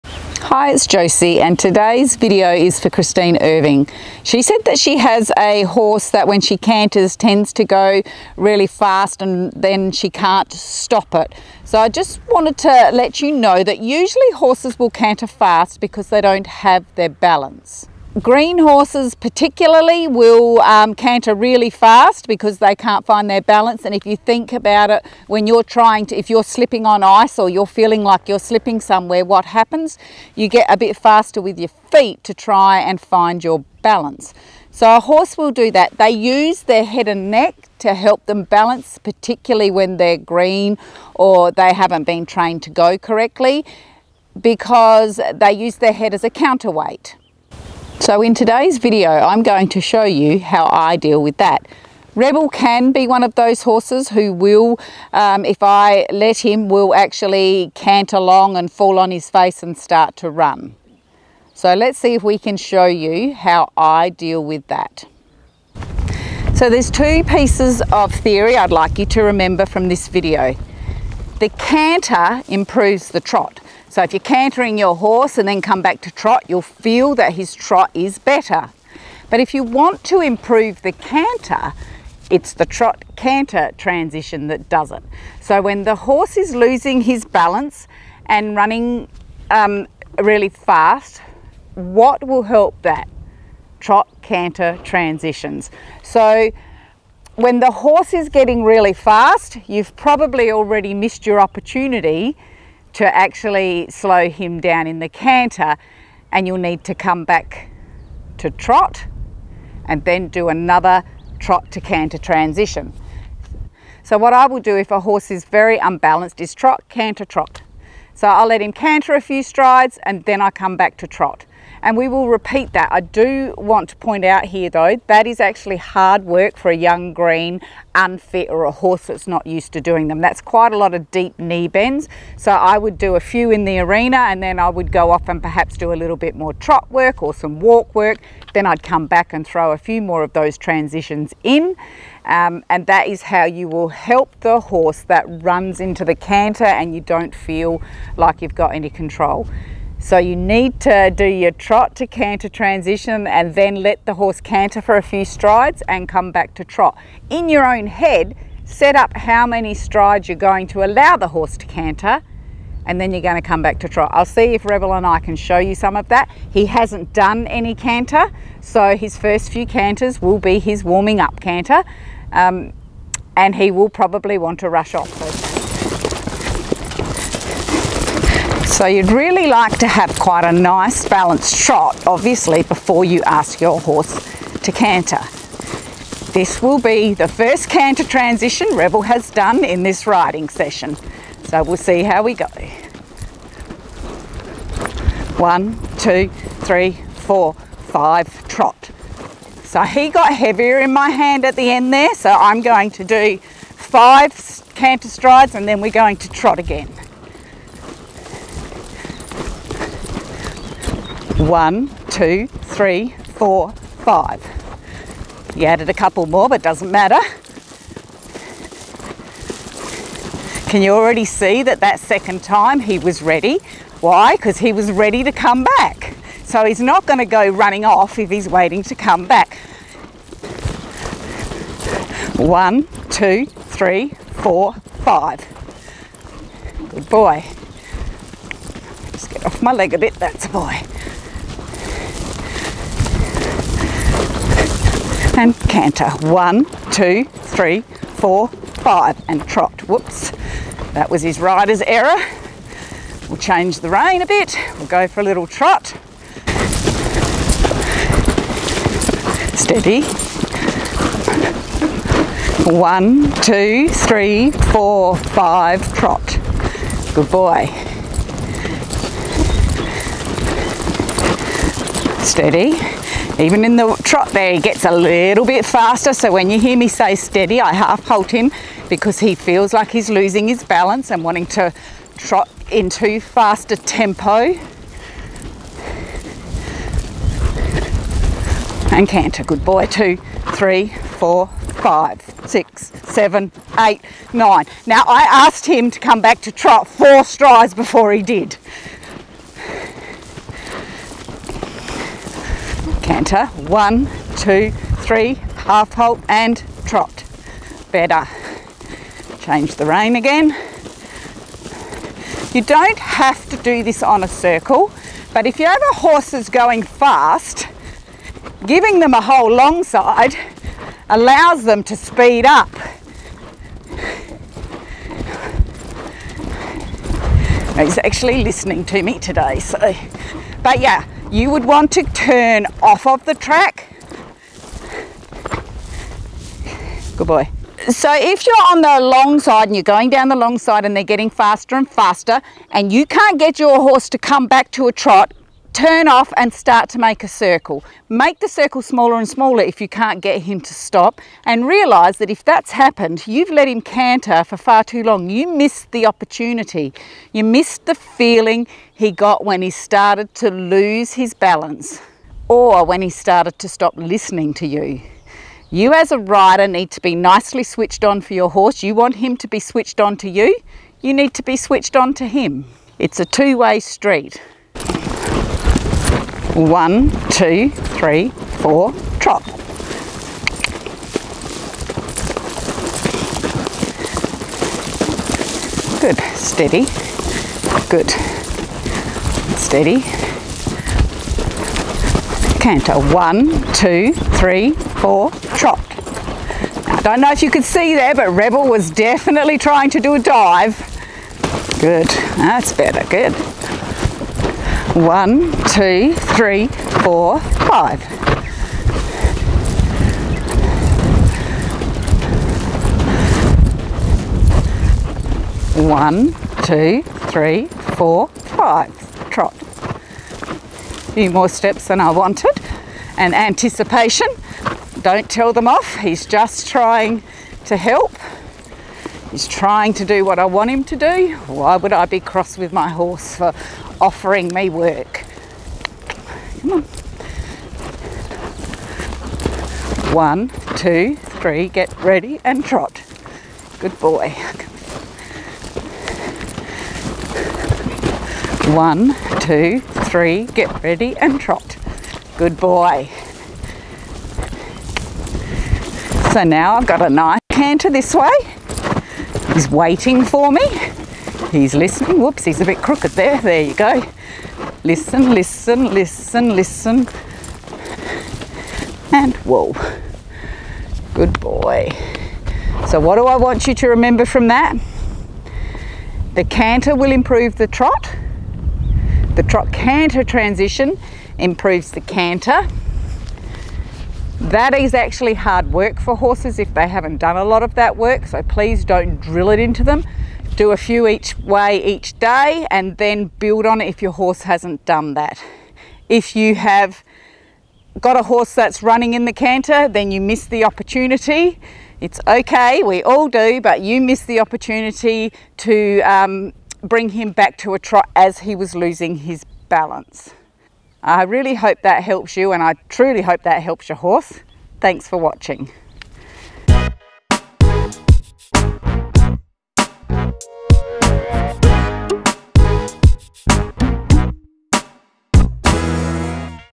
Cantering On A Horse That Gets Too Fast
When you are cantering on a horse and it gets too fast do you know what to do? Are your canter transitions horrible? Does your horse even know how to canter? If you are looking for some canter exercises for unbalanced horses then this video will help you and your horse. I show you how I help a horse to find his balance in the canter and how I slow one down that is going too fast. It is suitable for a green horse as well as the more advanced dressage horse.

Don't forget to download your FREE guide - 3 Principles to help you train your horse with ease: You will find it